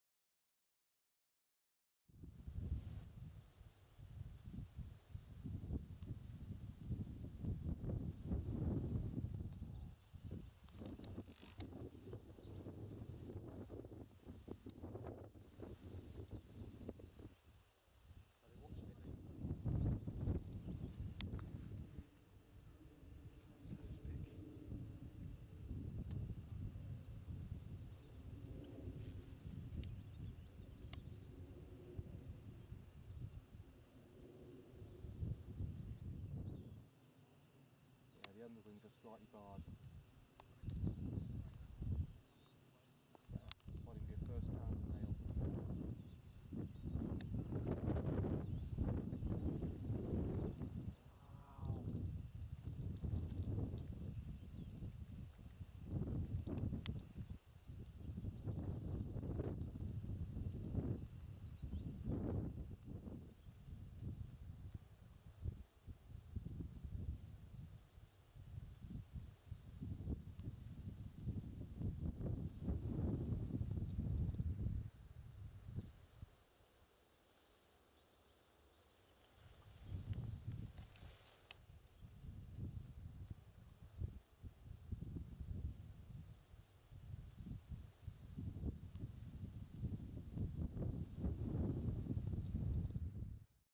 Red footed Falcon. The Lizard. Cornwall. 04/06/2017
A First summer Red-footed Falcon has been frequenting the Lizard over the past few days, After being seen briefly at Windmill Farm this Morning it was re-located at nearby Hayle Kimbro Pool where it remained for the afternoon. It wasn't an easy bird to keep up with, hence the short video